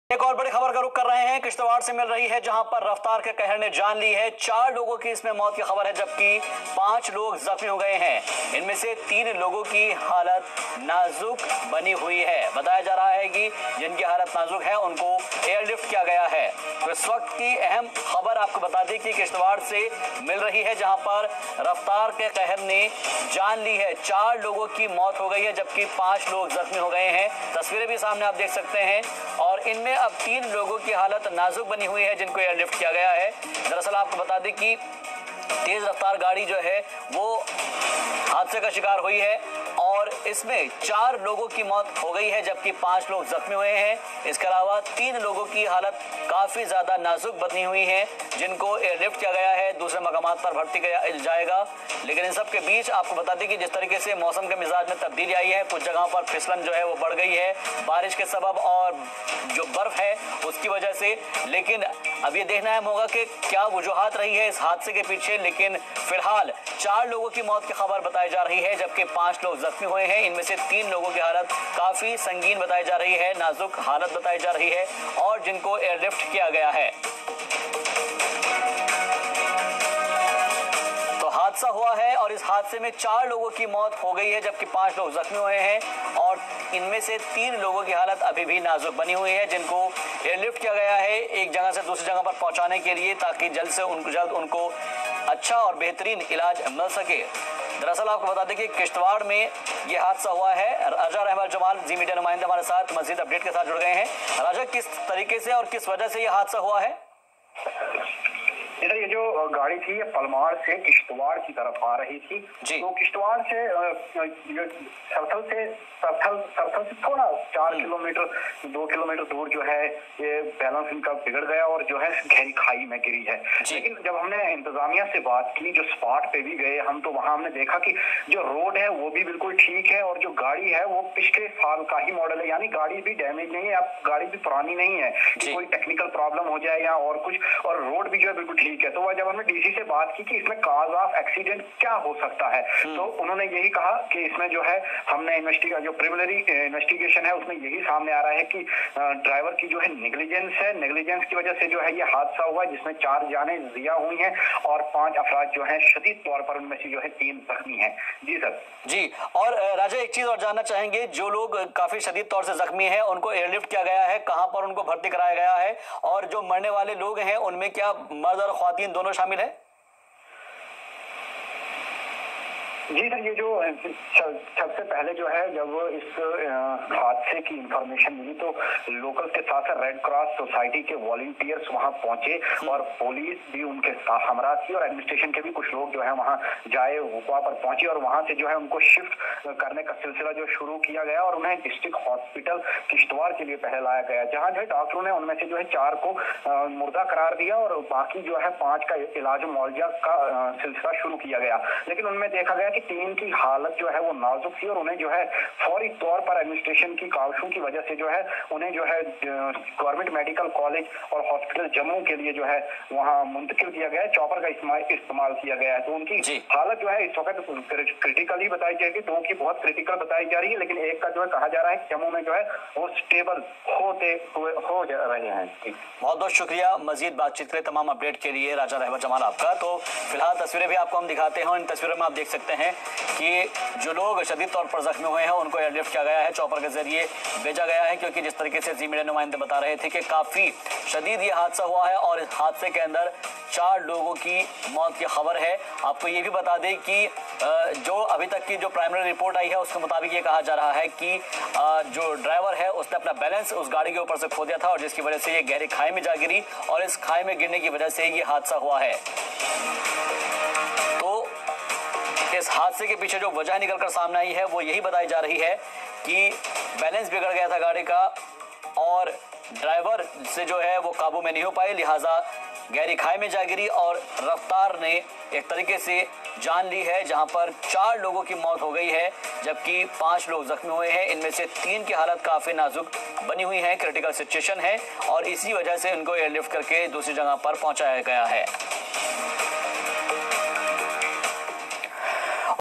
Full Report..Four dead, 5 injured in Cab accident near Kishtwar, 3 airlifted to GMC/ZEE
Four dead, 5 injured in Cab accident near Kishtwar, 3 airlifted to GMC